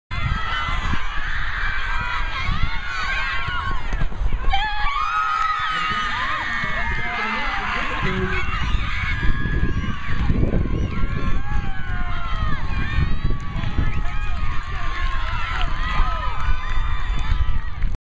Parkview Vikings vs Kickapoo Chiefs, McKenna Adams scores
Junior Adams scores on a breakaway, from senior Megan Melugin's flick on header inside the center circle.  Ties the game in the 76th min.